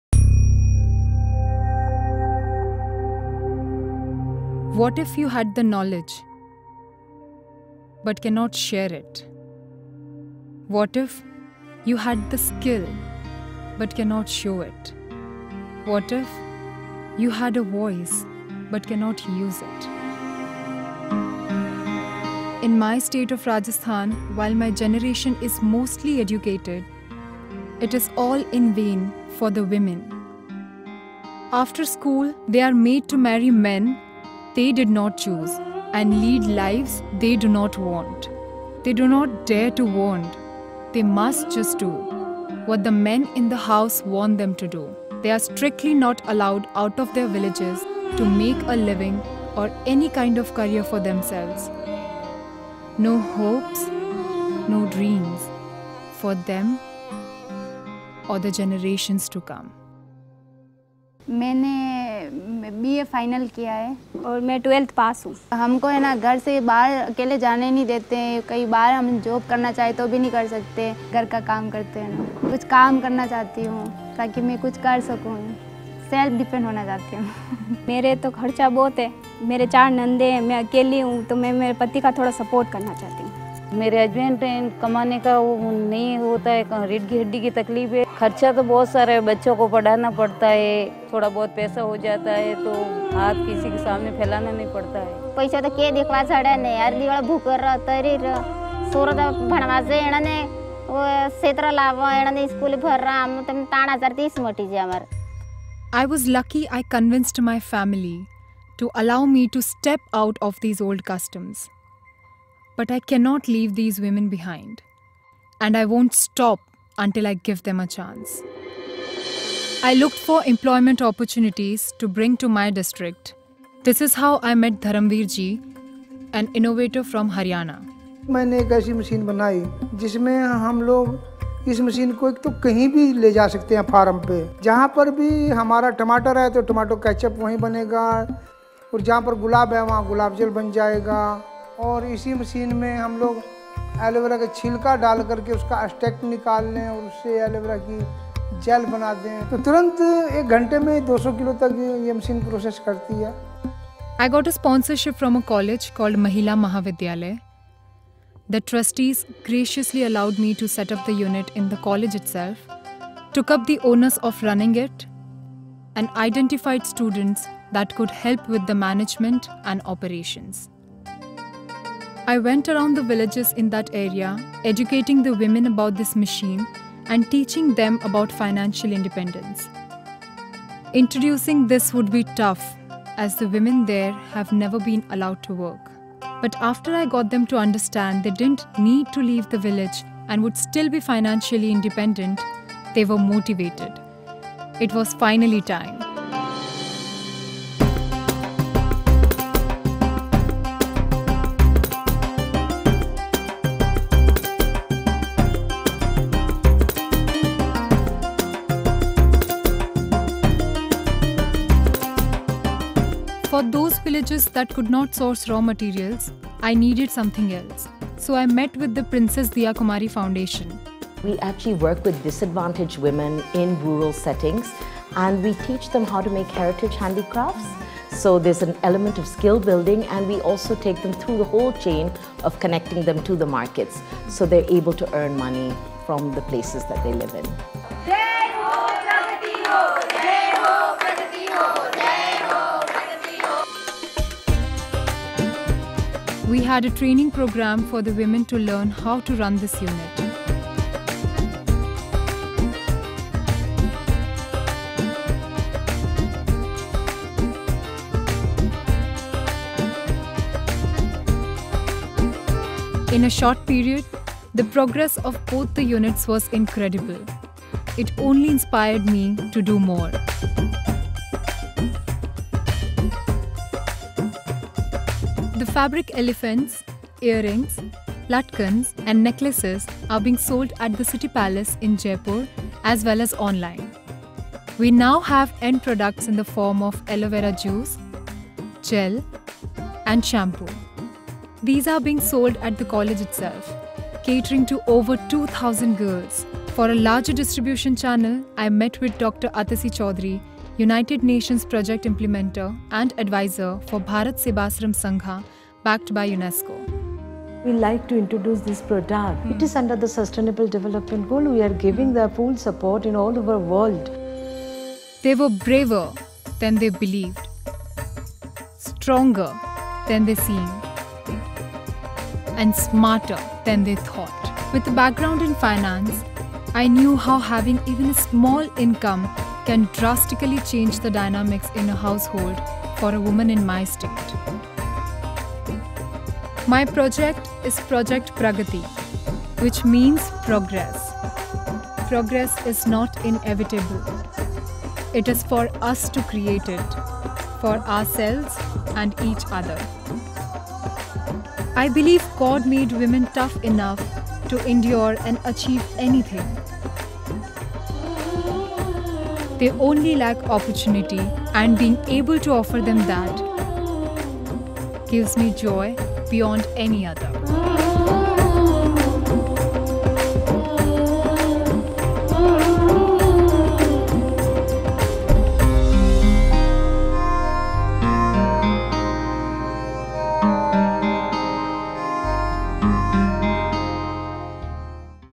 Suman Rao's BWAP project for Miss World 2019
fbb Colors Femina Miss World 2019 Suman Rao's BWAP project for Miss World 2019.

Suman Rao's Mission: Women living in the tribal areas have fewer opportunities to earn and to be able to live a better standard of living. ProjectPragati aims at creating employment opportunities with the help of resources available around their area and by putting to use their skill sets.